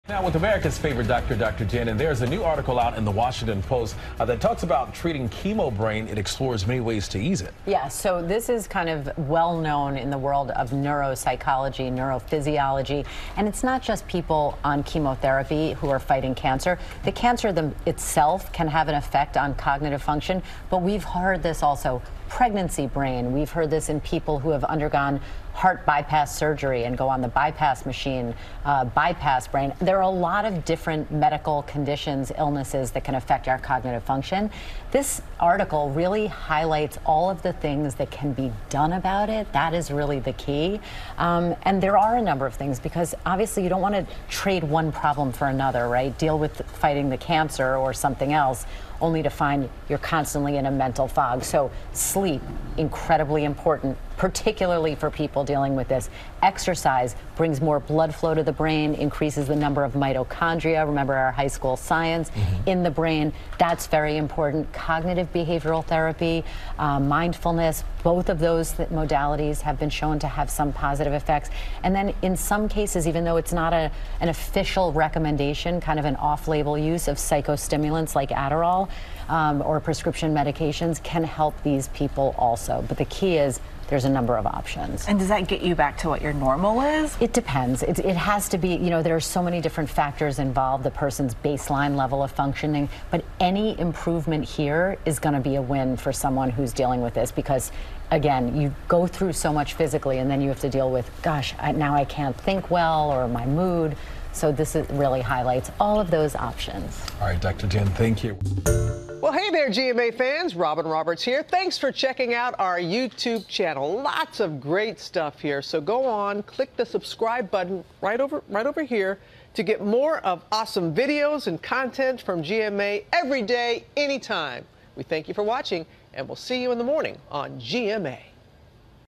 Tips to treat chemo brain
ABC News Chief Medical Correspondent Dr. Jen Ashton explains a study on ways to provide relief for chemo brain, which involves multi-pronged approach according to neuropsychologists.